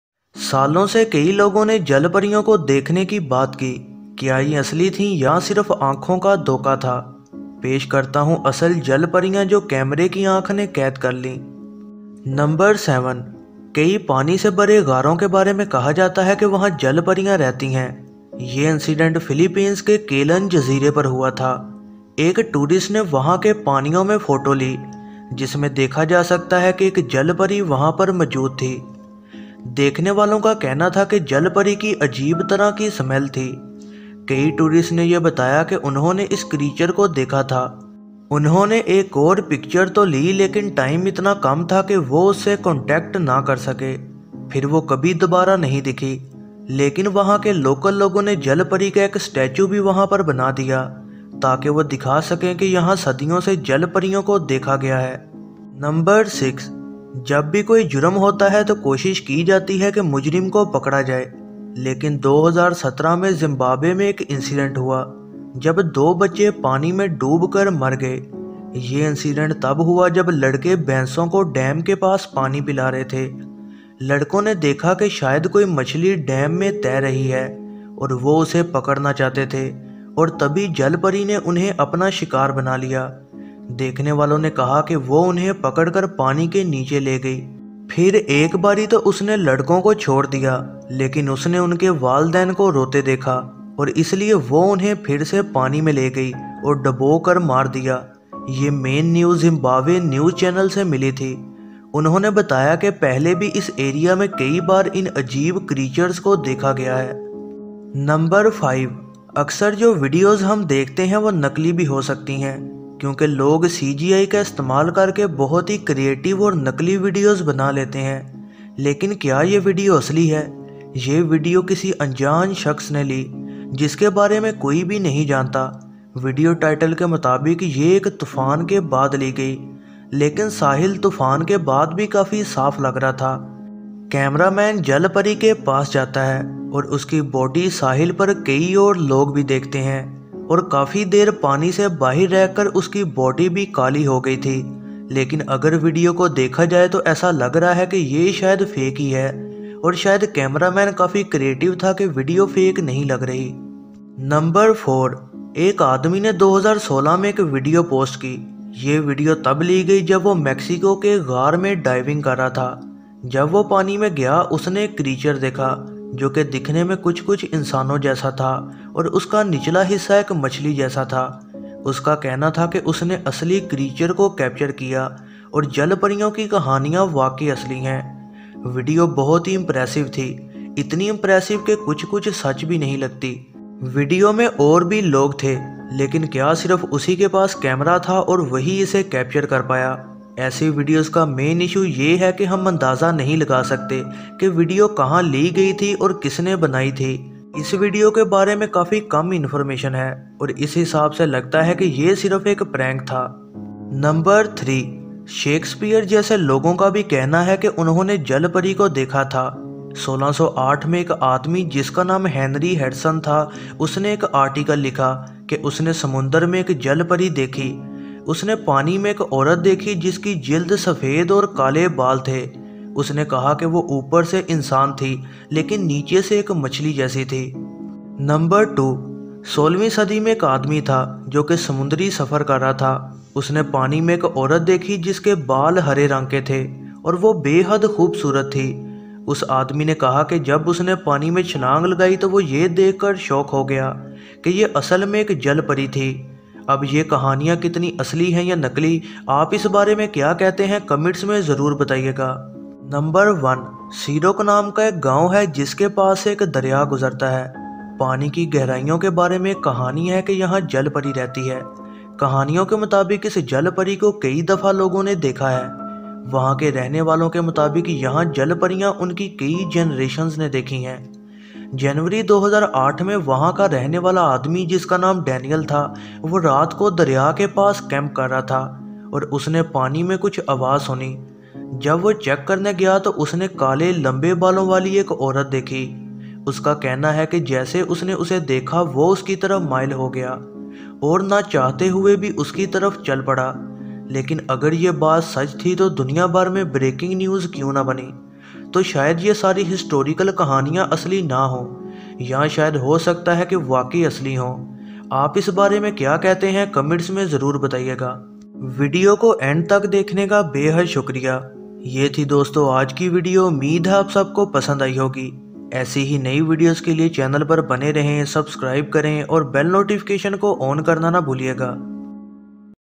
7 Real Life Mermaid Sightings From History In Urdu / Hindi
7 Real Life Mermaid Sightings From History In Urdu / Hindi
                                  7 Real Life Mermaid Sightings caught on camera and spotted in real life that are mentioned on the news and history .